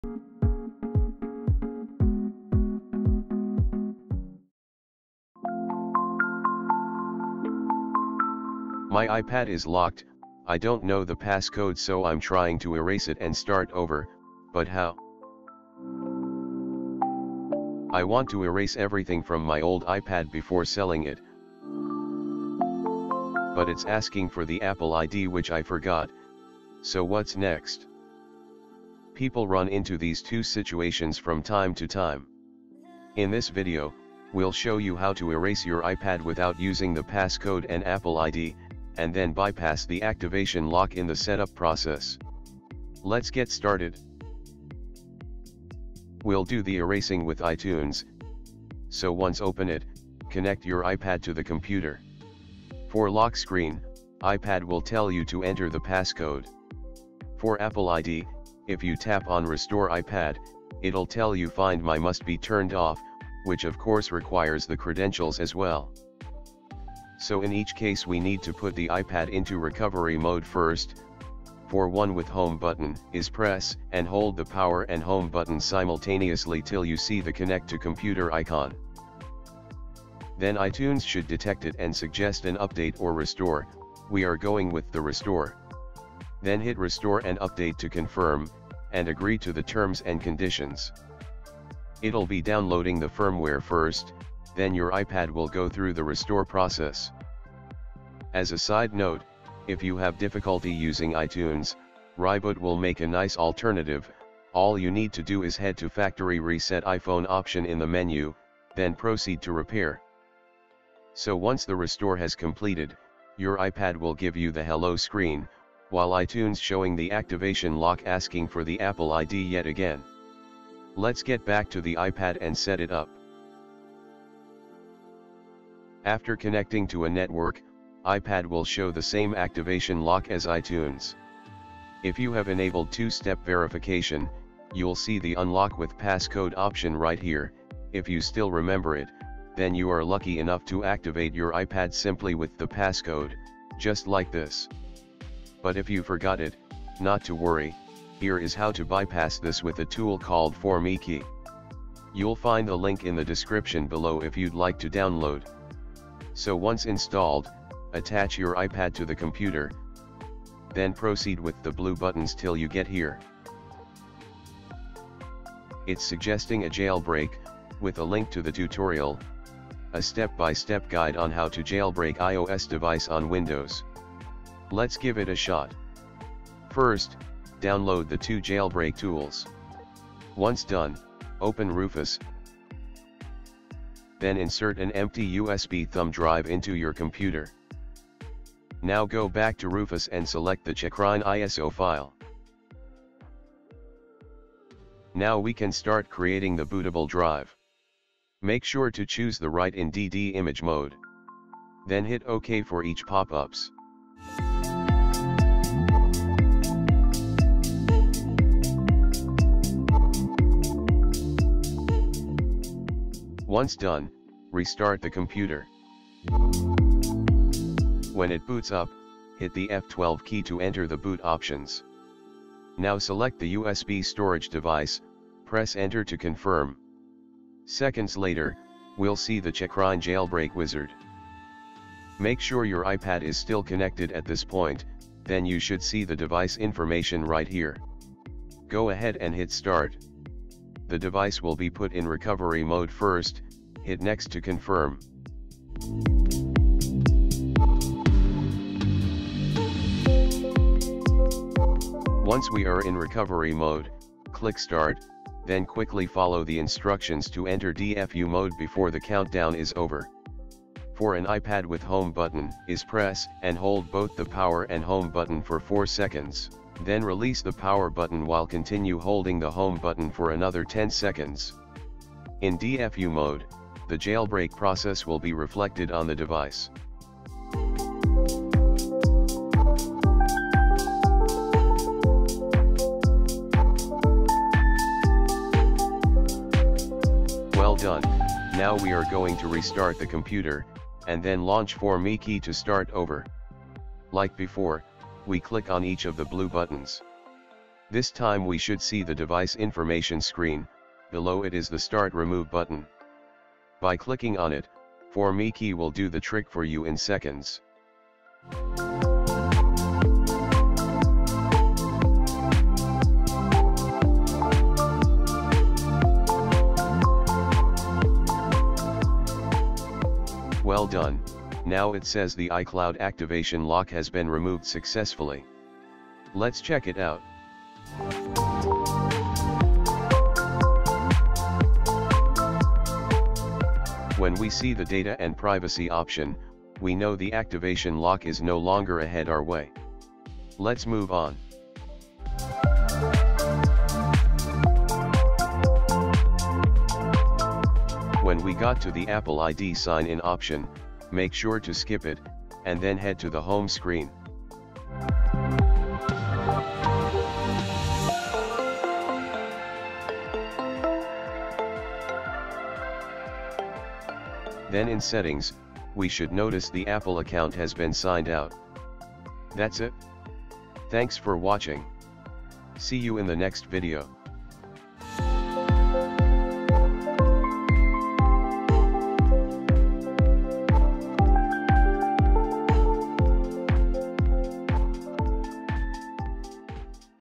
How to Restore iPad without Apple ID or Password If Forgot
You’d want to restore your iPad to factory settigns if you forgot the passcode, want to sell it or give it to your friend. The thing is, if you forgot the Apple ID while Find My is on, iPad won’t allow you to erase it and iTunes will give you Activation Lock. In this video, we’ll show you how to pull it off without using the passcode, Apple ID, and bypass the Activation Lock afterwards.

⚠Important notice:
2. 4MeKey is only designed to help individual users who forgot their Apple ID or Apple ID password, commercial use is not permitted.
3. This tool is unable to obtain access to credentials, compromise personal data or cause serious harm to others. Do not try to violate YouTube community guidelines.
4. This tool will jailbreak your iOS device before removing iCloud Activation Lock or turning off Find My iPhone.
5. Once succeeded, you are unable to use the phone for the function of phone call, cellular and iCloud with your Apple ID. However, you can log in a new Apple ID through App Store in Settings and install Apps in App Store.
6. This tool currently only supports iOS devices with A7 to A11 Bionic running iOS 12 to iOS 14.
7. Restarting, updating and restoring the device to factory resettings after the removal will bring the device back to the Activation Lock screen.

Timestamps:
0:00 Intro and preview.
0:47 Step 1 - Erase iPad using iTunes.
2:32 Step 2 - Bypass Activation Lock with 4MeKey.
2:55 Jailbreak.
4:51 Start removing.
5:24 Activation Lock bypassed successfully.

📖Related article:
4 Easy Ways To Factory Reset iPad Without Apple ID And Password 2024